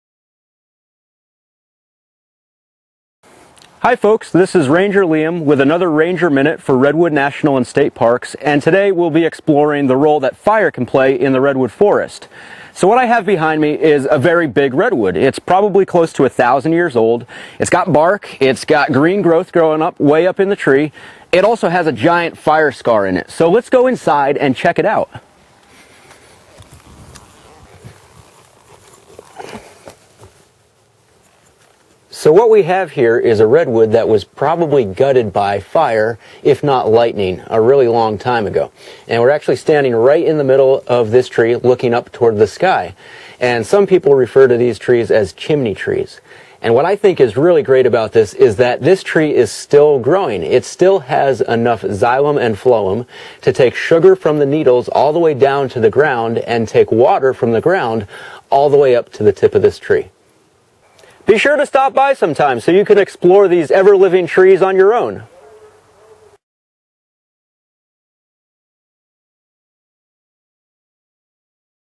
Redwood Ranger Minute: Chimney Trees
Direct from Redwood National and State Parks, Ranger Liam takes you inside a living coast redwood.